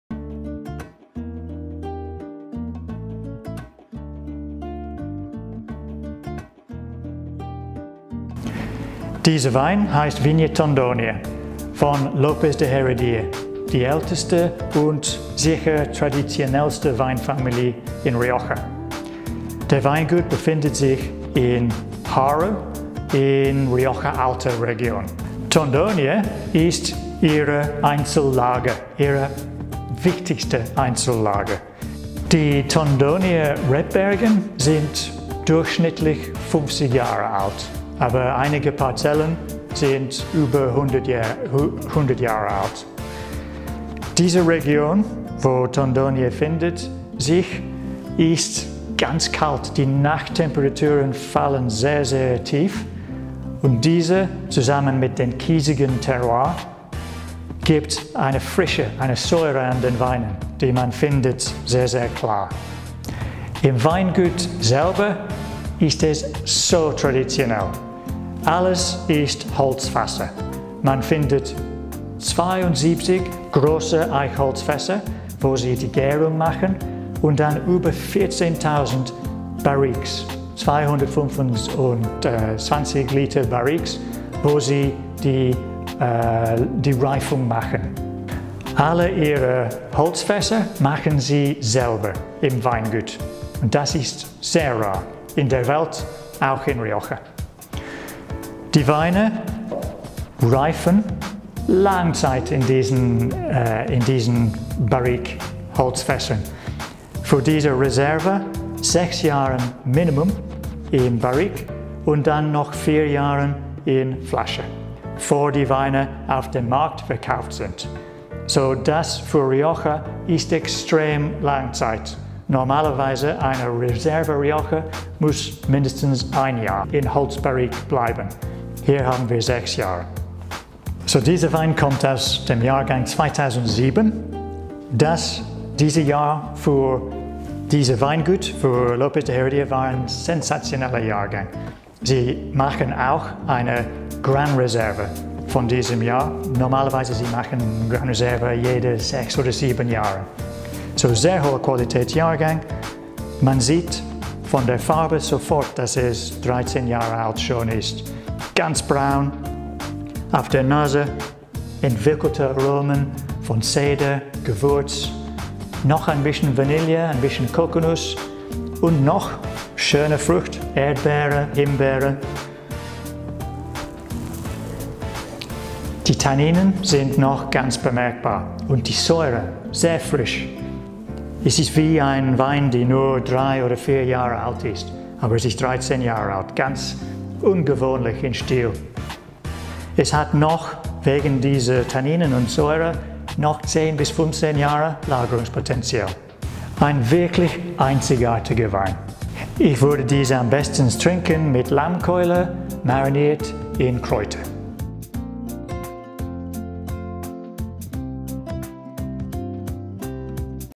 Rioja´s Best - Vina Tondonia Reserva 2007
Der Vina Tondoñia Reserva 2007 von der Bodegas López de Heredia, der ältesten und traditionellsten Weinfamilie aus der Rioja, wird Euch vom Master of Wine Paul Liversedge präsentiert.